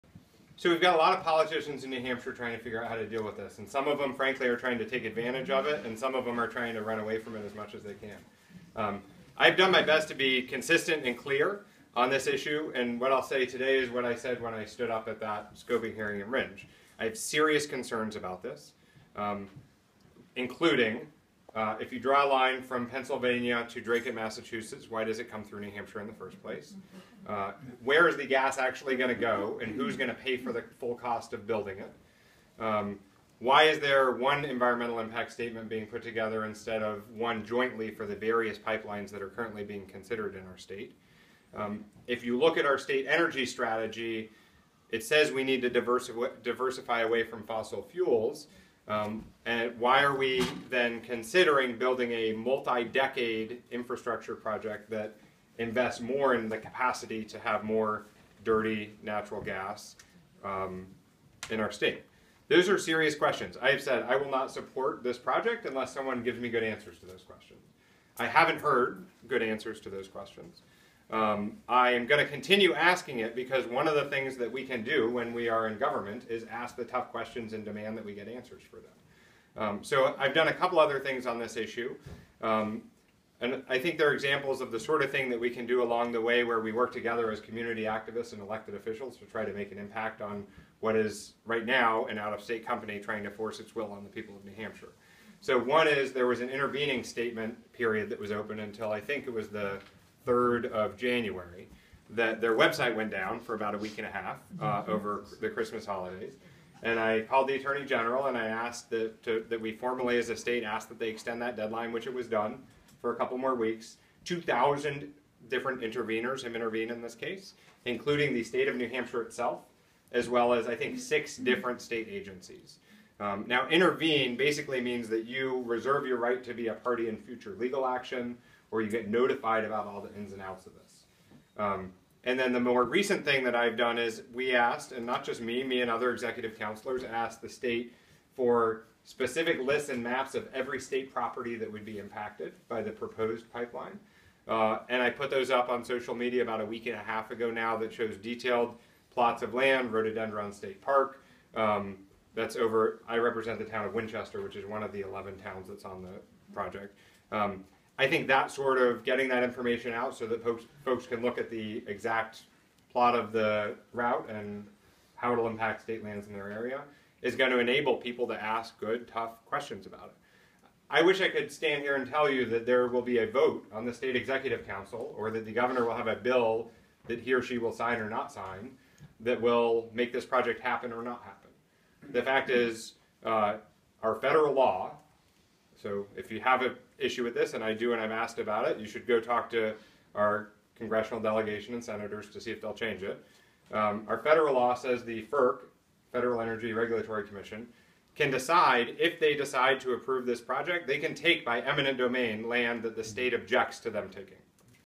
Van Ostern on the NED pipeline
Colin Van Ostern, gubernatorial candidate in NH, lays out his views on Kinder Morgan's proposed fracked gas pipeline to a group of citizens from Temple, Greenville, New Ipswich, Fitzwilliam, Mason, Sharon, Peterborough, Acworth, etc. This took place at a house party in Temple, NH on Jan. 31, 2016.